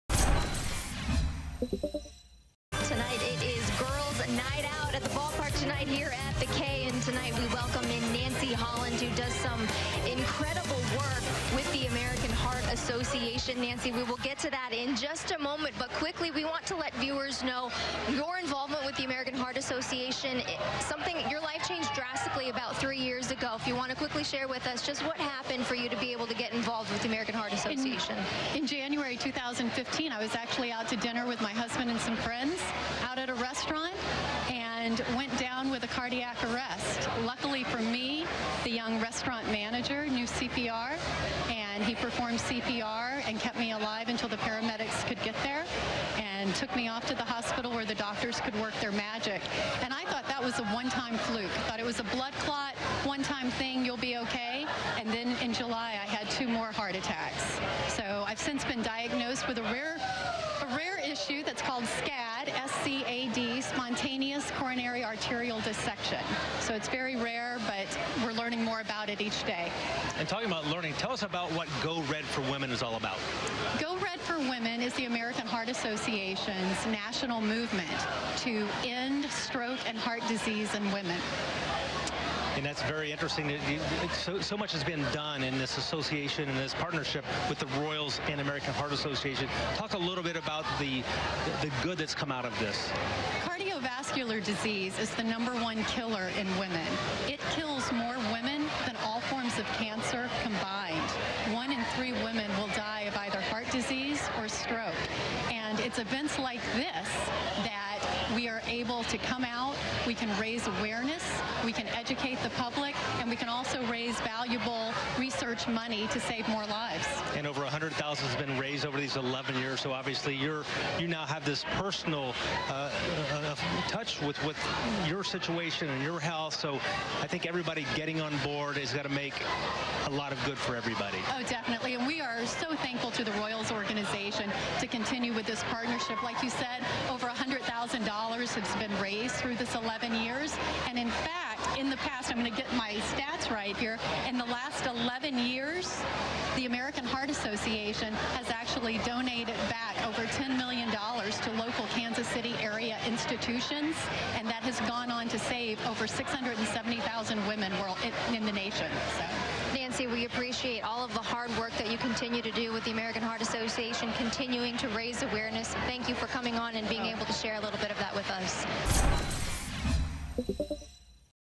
Royals look to help end heart disease and strokes in women
The Kansas City Royals partner up with the American Heart Association.